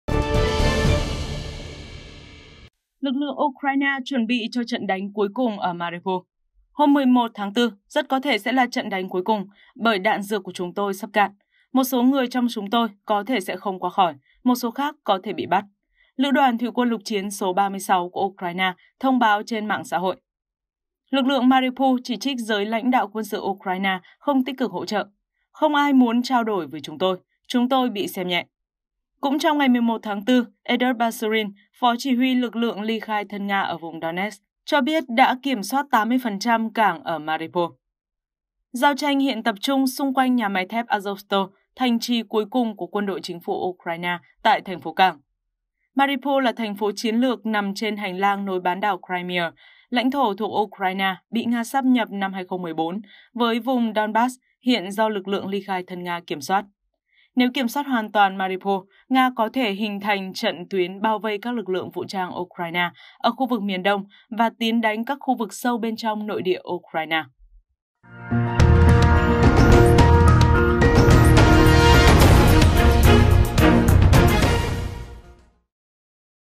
Lực lượng Ukraine chuẩn bị cho 'trận đánh cuối cùng' ở Mariupol | Thời Sự TV
Lữ đoàn thủy quân lục chiến Ukraine số 36, đơn vị đang bảo vệ thành phố cảng Mariupol, cho biết sắp hết đạn dược nhưng vẫn kiên định chuẩn bị đối mặt với trận đánh 'cuối cùng'.